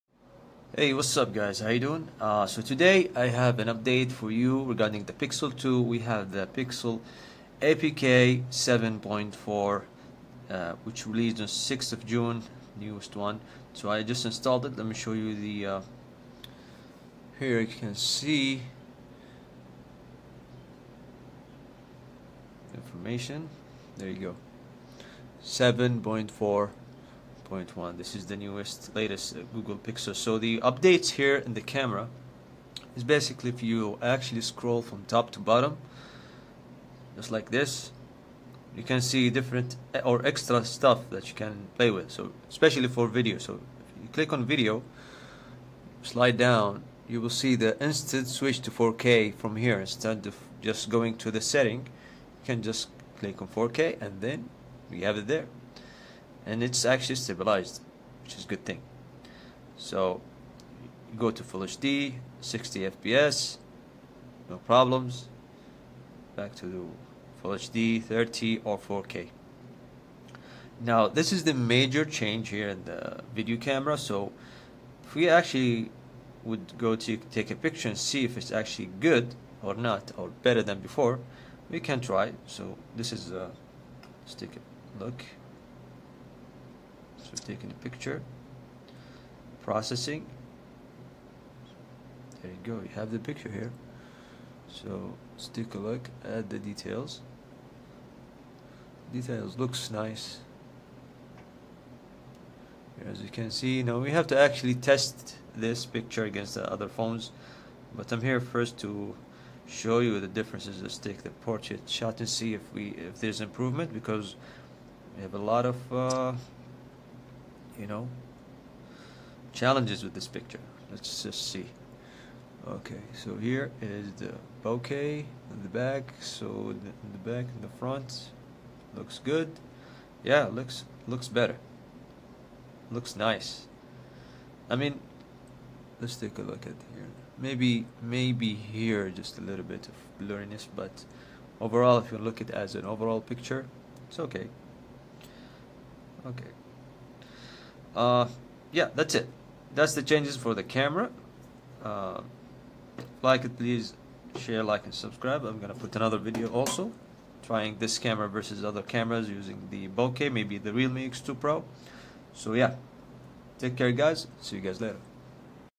Camera Update : Gcam 7.4 on Pixel 2
Let's take a look at this update from Google, minor changes but good one.